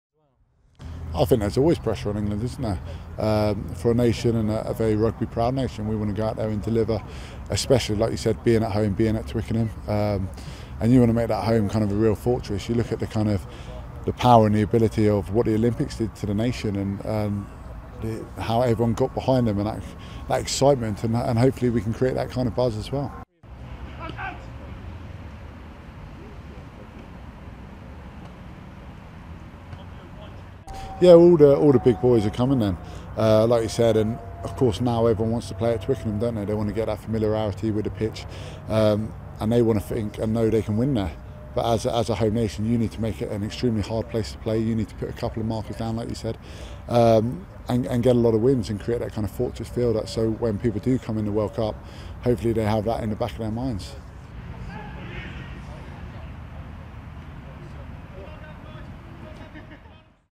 Twickenham can be 'fortress' - Robshaw [AMBIENT]
England captain and Harlequins flanker Chris Robshaw says the spirit of London 2012 can inspire the hosts to glory in the 2015 Rugby World Cup.